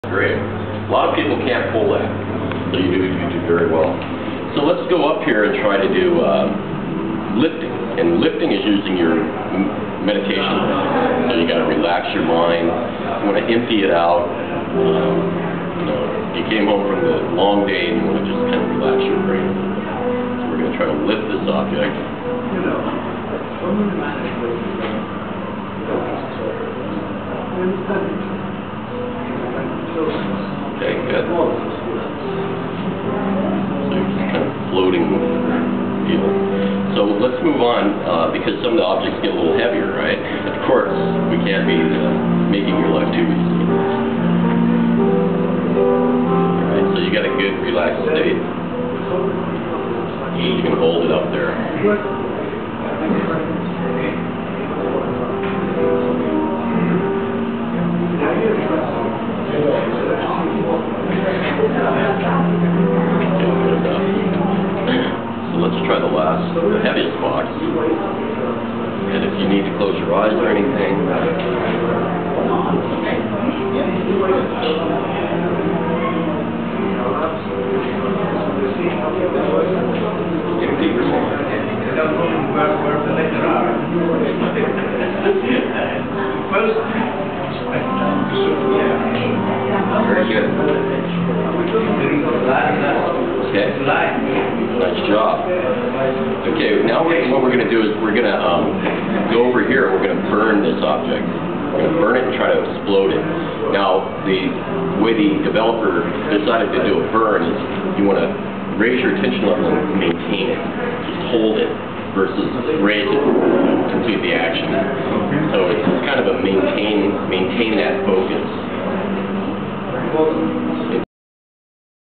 NeuroSky brain controlled interface at View Conference 2008
Software composto da un cuffia avente un dispositivo che appoggiandosi sulla fronte questo capta le onde elettromagnetiche del cervello.
Nel "gioco" del video il personaggino può sollevare e spostare oggetti.